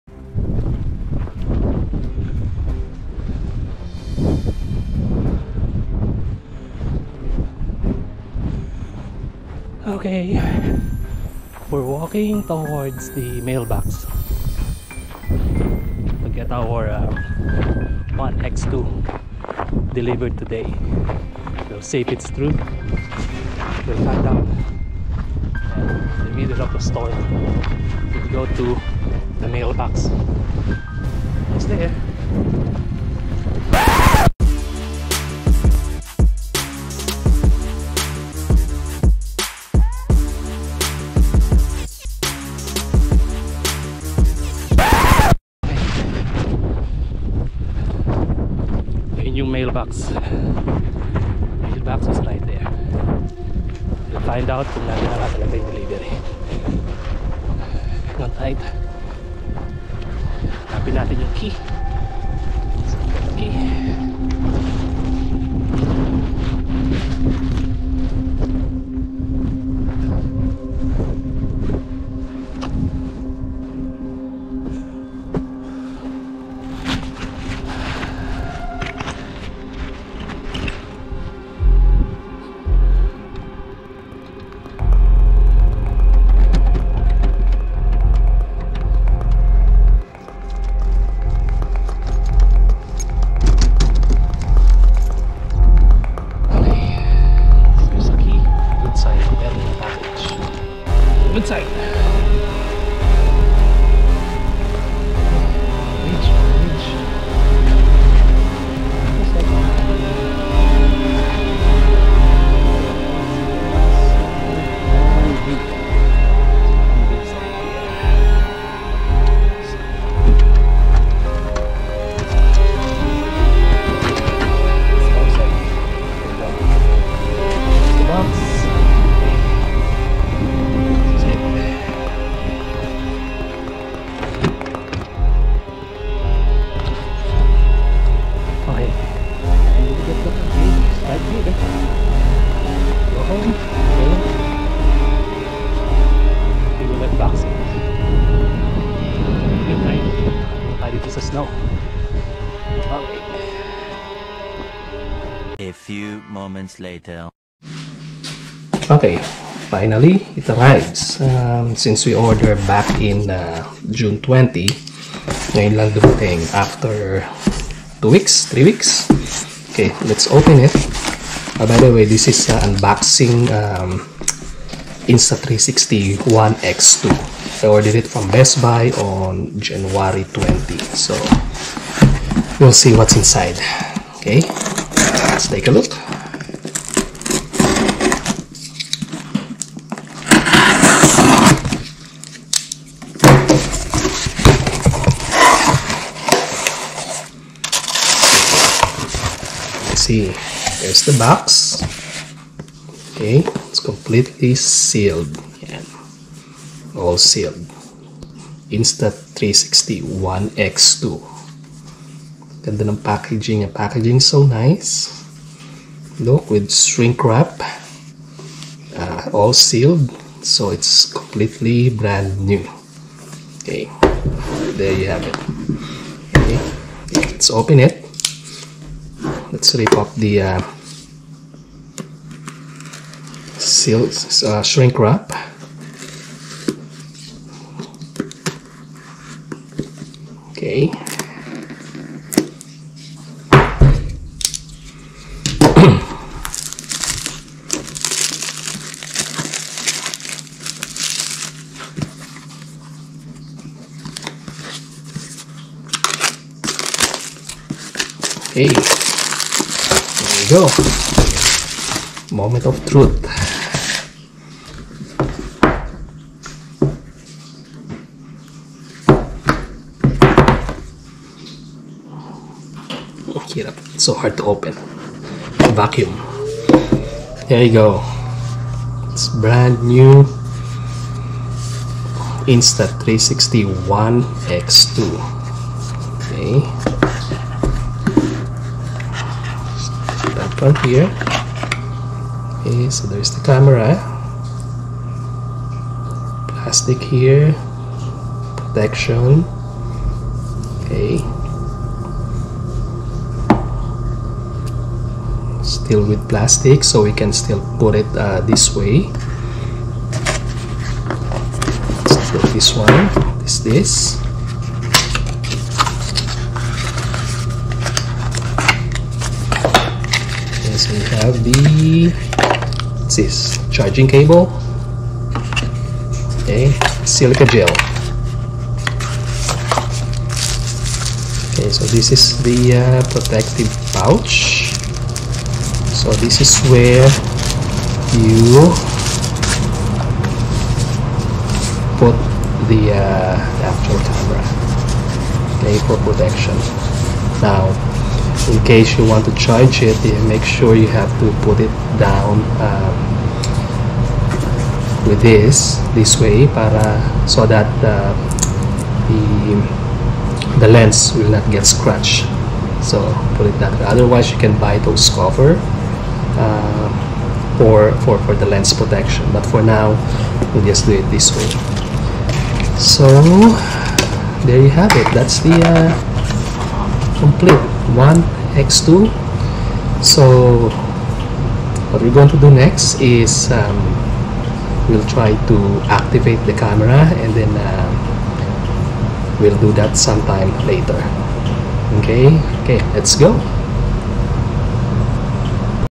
UNBOXING: INSTA360 ONE X2
From the journey to the mailbox on a snowy weather to unboxing the Insta360 One X2.